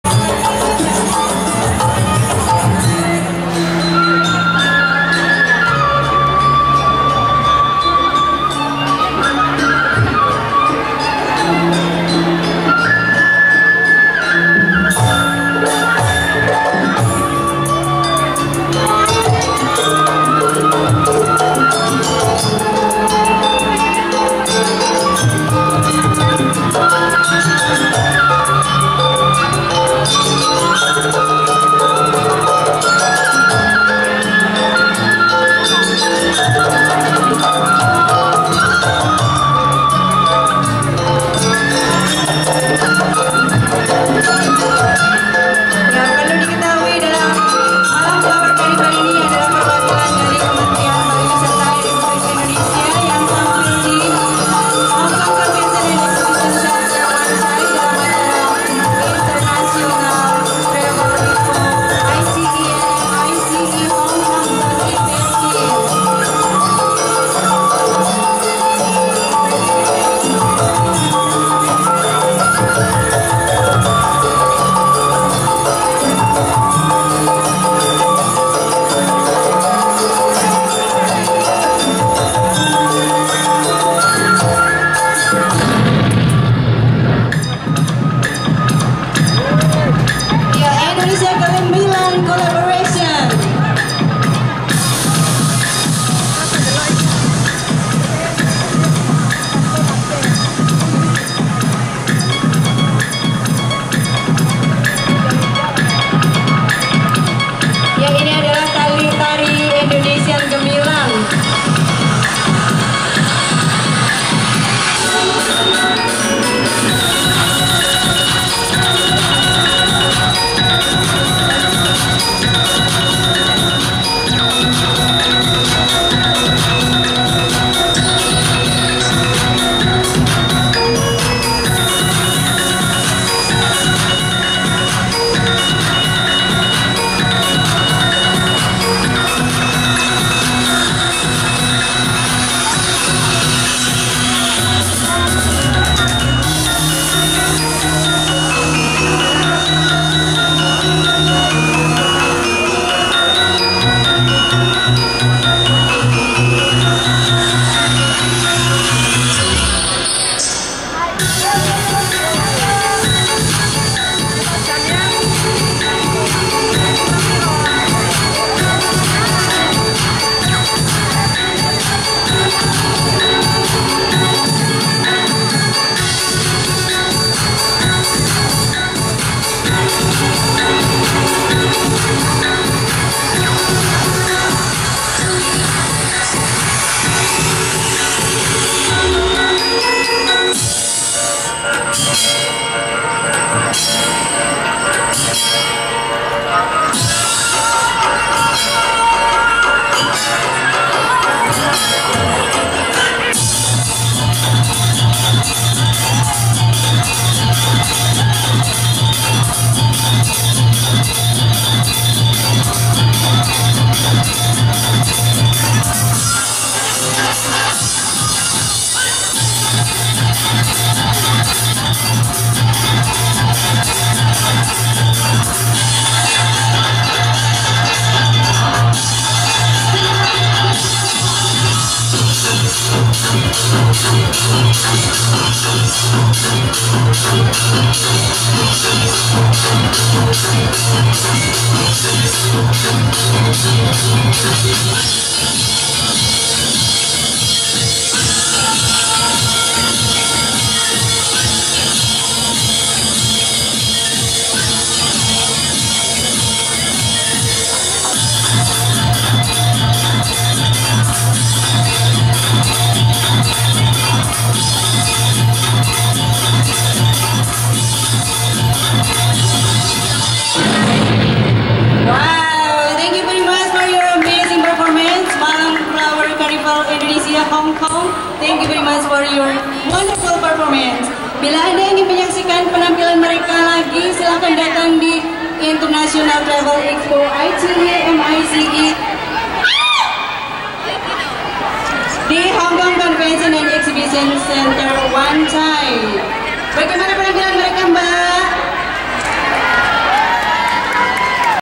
MENAKJUBKAN PENAMPILAN KREASI SENI BUDAYA INDONESIA DI HONGKONG(JEAND82)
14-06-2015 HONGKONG